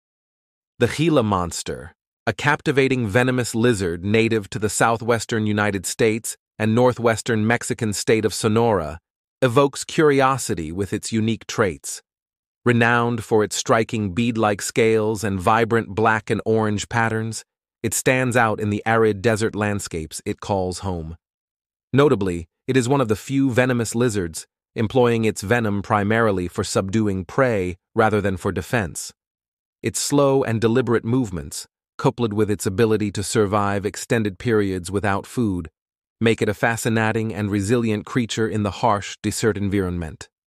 ''The Gila Monster: A Desert Marvel🔥''
🌵 Explore the captivating world of the Gila monster, a venomous lizard that thrives in the arid desert landscapes of the southwestern United States and the northwestern Mexican state of Sonora.
Marvel at its distinctive features, including its striking bead-like scales and vibrant black and orange patterns, which make it an iconic symbol of the desert wilderness. Discover the intriguing behaviors of this unique reptile, known for using its venom primarily for subduing prey, and observe its deliberate movements and remarkable adaptability to the challenges of the harsh desert environment. Delve into the secrets of this resilient creature, unveiling its fascinating survival strategies and its essential role in the delicate balance of the desert ecosystem.